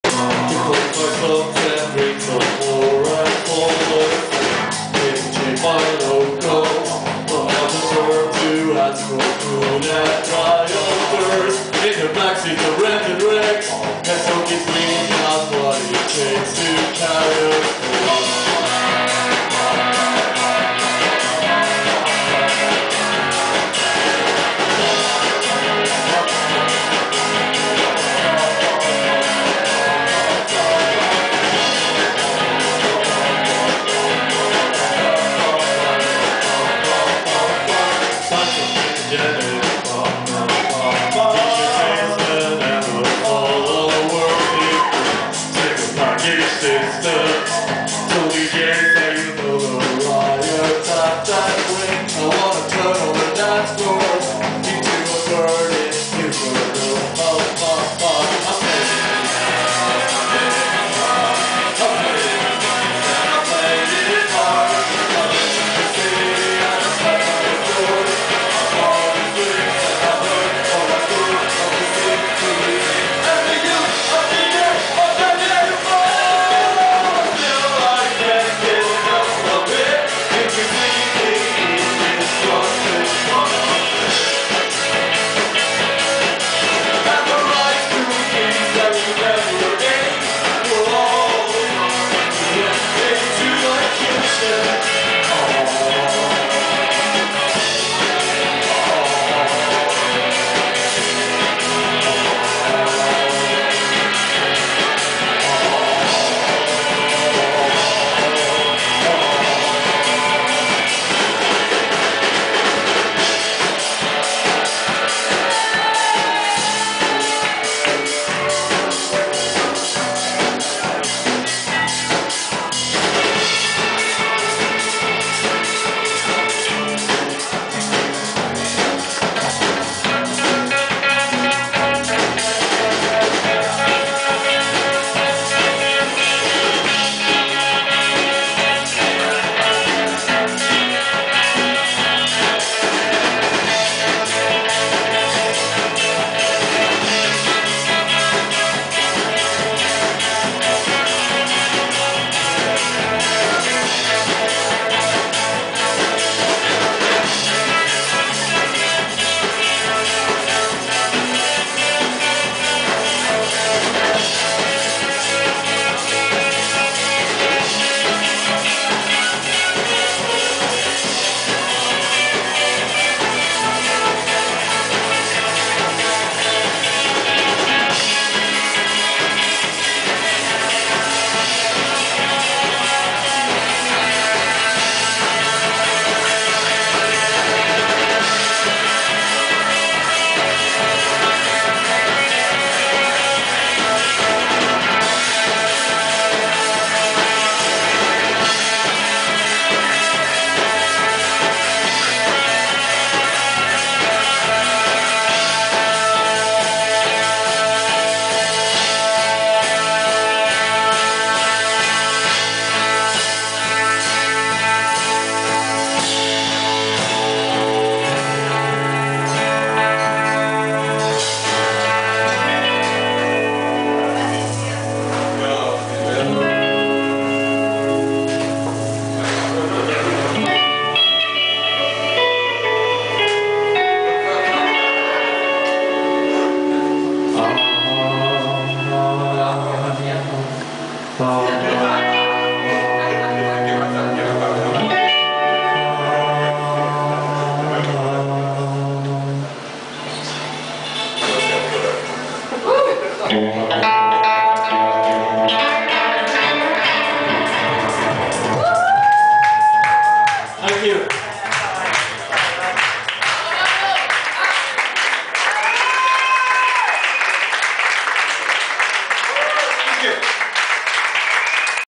Suburban Kids With Biblical Names @ Fotomatón Bar (Madrid) 25 mayo, 2010
Suburban Kids With Biblical Names
directo (live) @ Fotomatón Bar (Madrid, Spain)
25 mayo, 2010
Organizado por Fikasound